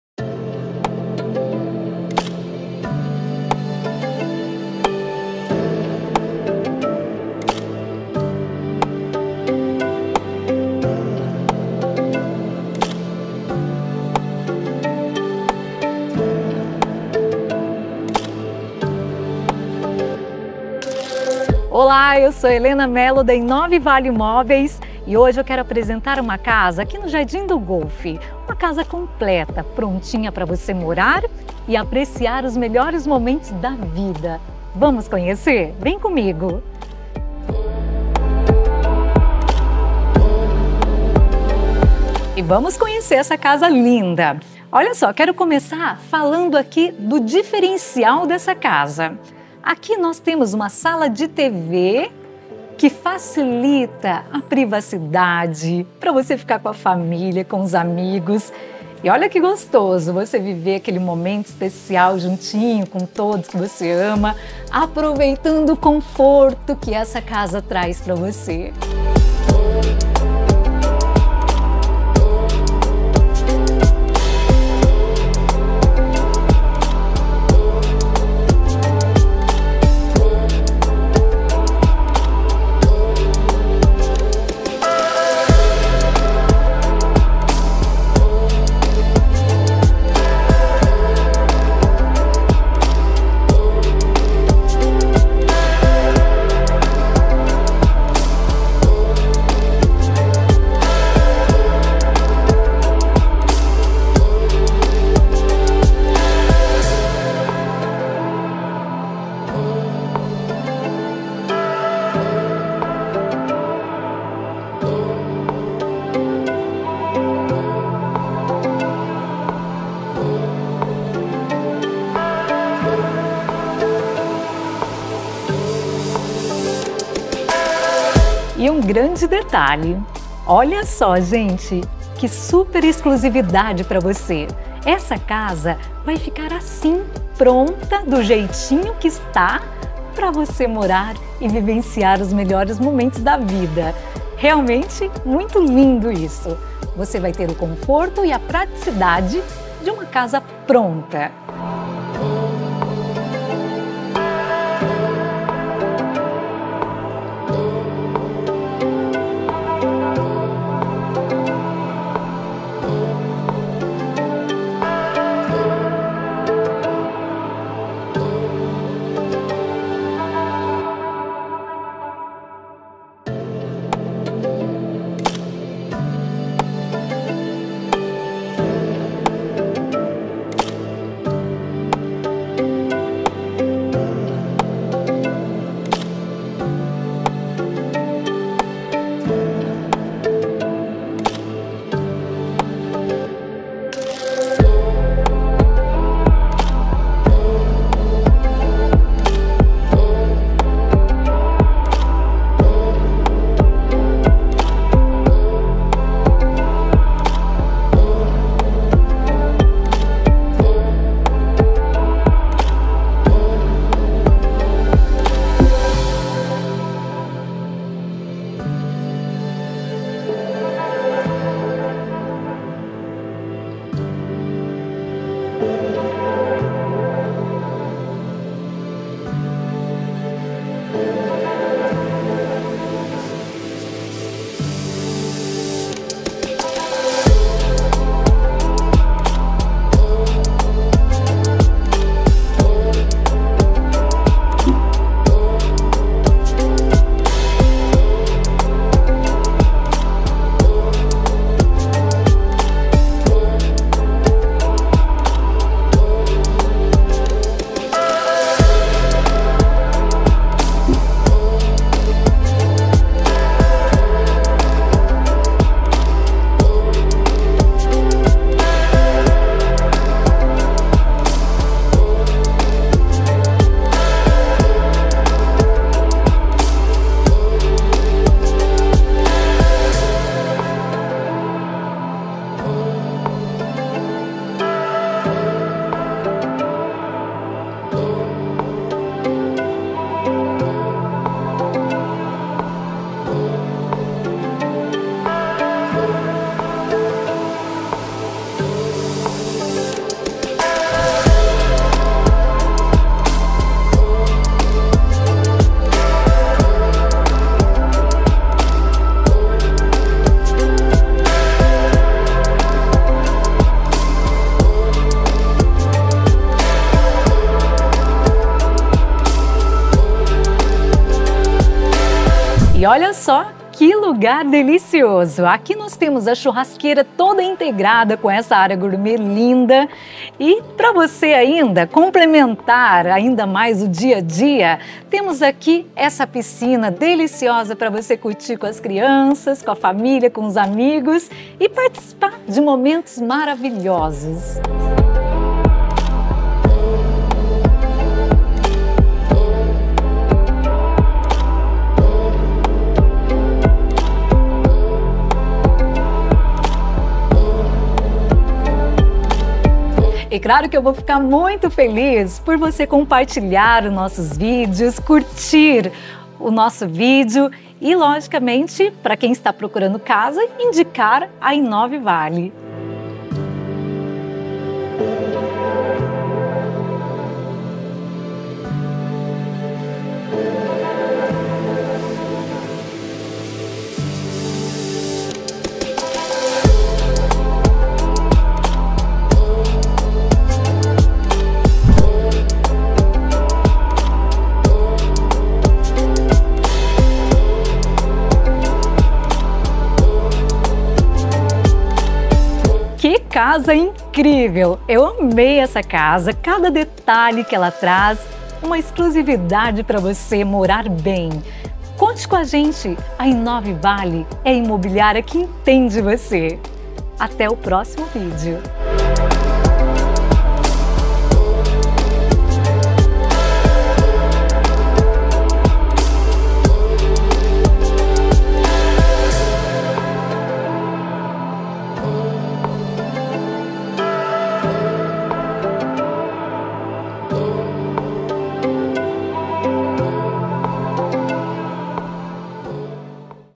Casa luxuosa no condomínio Jardim do Golfe em São José dos Campos | i9vale Imoveis luxo SJC
Decoração moderna feita por arquiteta renomada, Todos moveis são de lojas renomadas Butique da casa e Breton, Tapetes Revolution - by Kamy, Home completo com telão automatizado, Sendo 5 quartos com uma suíte no piso térreo 3 suítes no piso superior sendo um quarto atendendo a familia como escritório.
Varanda gourmet toda integrada com cozinha e área da churrasqueira, juntamente com a piscina , além de tudo a casa ja esta com fotovoltaica, circuito de câmeras, piscina aquecida e energia solar aproveite essa oportunidade e traga só as malas.